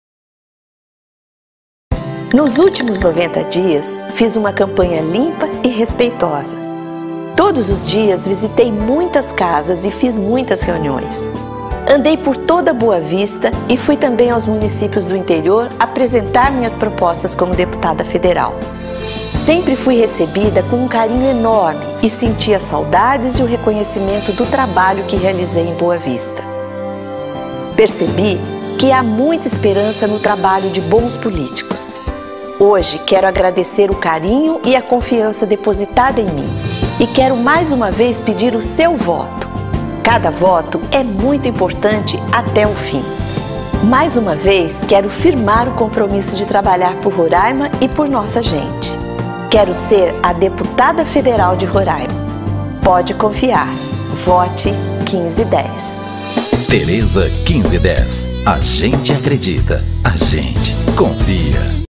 Teresa Surita Deputada Federal 1510 - Nossa deputada federal
teresasurita.com - Este é o último programa da candidata a deputada federal Teresa 1510 exibido na propaganda eleitoral gratuita. No programa, Teresa Surita agradece todo o carinho recebido durante estes três meses de campanha e reafirma o compromisso com a população de Boa Vista e Roraima.